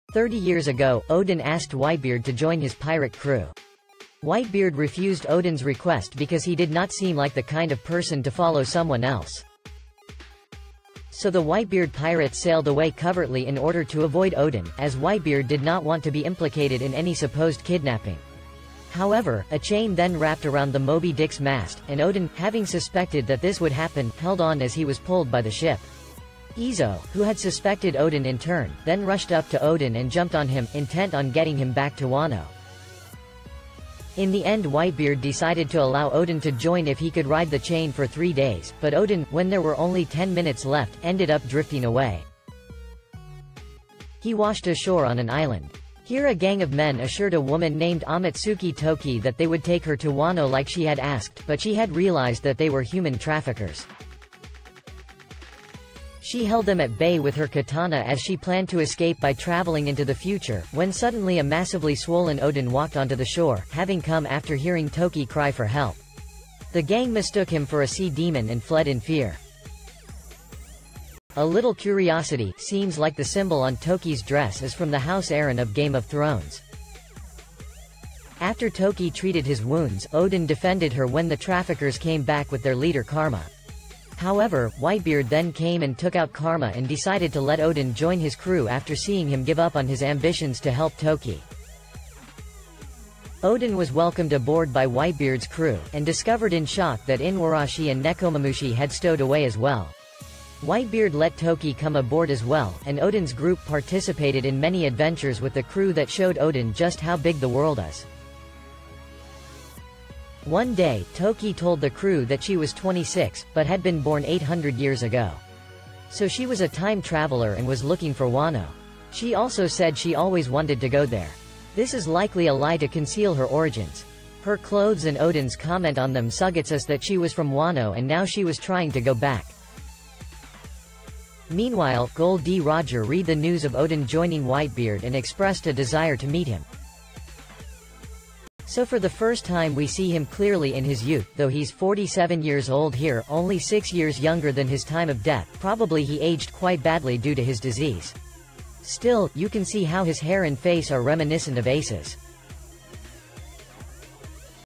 The Adventures Oden & Toki with Whitebeard
The Adventures Oden & Toki with Whitebeard. Theories One Piece and Anime. 30 years ago, Oden asked Whitebeard to join his pirate crew. Whitebeard refused Oden’s request because he did not seem like the kind of person to follow someone else.
So the Whitebeard Pirates sailed away covertly in order to avoid Oden, as Whitebeard did not want to be implicated in any supposed kidnapping. However, a chain then wrapped around the Moby Dick’s mast, and Oden, having suspected that this would happen, held on as he was pulled by the ship. Izo, who had suspected Oden in turn, then rushed up to Oden and jumped on him, intent on getting him back to Wano.
In the end Whitebeard decided to allow Oden to join if he could ride the chain for three days, but Oden, when there were only ten minutes left, ended up drifting away.
He washed ashore on an island. Here a gang of men assured a woman named Amatsuki Toki that they would take her to Wano like she had asked, but she had realized that they were human traffickers.
She held them at bay with her katana as she planned to escape by traveling into the future, when suddenly a massively swollen Oden walked onto the shore, having come after hearing Toki cry for help. The gang mistook him for a sea demon and fled in fear.
A little curiosity: Seems like the symbol on Toki’s dress is from the House Arryn of Game of Thrones.
After Toki treated his wounds, Oden defended her when the traffickers came back with their leader Karma. However, Whitebeard then came and took out Karma and decided to let Oden join his crew after seeing him give up on his ambitions to help Toki.
Oden was welcomed aboard by Whitebeard’s crew, and discovered in shock that Inuarashi and Nekomamushi had stowed away as well. Whitebeard let Toki come aboard as well, and Oden’s group participated in many adventures with the crew that showed Oden just how big the world is.
One day, Toki told the crew that she was 26, but had been born 800 years ago. So she was a time traveler and was looking for Wano. She also said she always wanted to go there. This is likely a lie to conceal her origins. Her clothes and Oden’s comment on them suggets us that she was from Wano and now she was trying to go back.
Meanwhile, Gol D. Roger read the news of Oden joining Whitebeard and expressed a desire to meet him.
So for the first time we see him clearly in his youth, though he’s 47 years old here, only 6 years younger than his time of death.. probably he aged quite badly due to his disease. Still, you can see how his hair and face are reminiscent of Ace’s! .